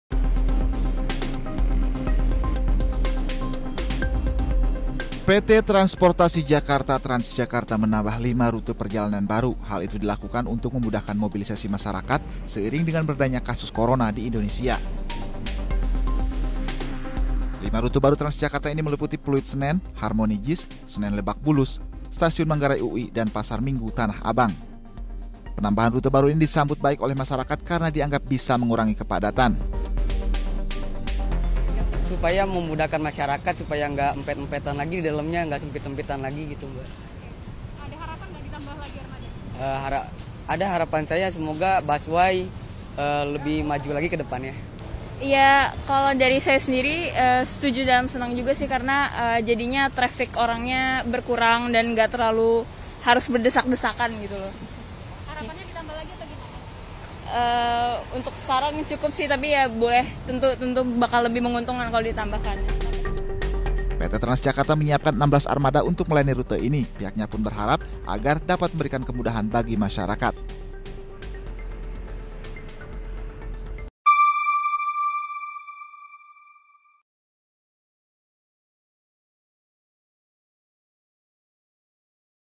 Rute Baru Transjakarta Armada Bus Ditambah untuk 5 Rute Perjalanan
Mulai hari Senin (20/6) ini, PT Tranportasi Jakarta menambah armada bus untuk 5 rute perjalanan. Hal ini diharapkan bisa memberikan kemudahan bagi masyarakat yang menggunakan transportasi tersebut.

Liputan: Esra Ambarita/Edmon Dantes/Abdul Rosyid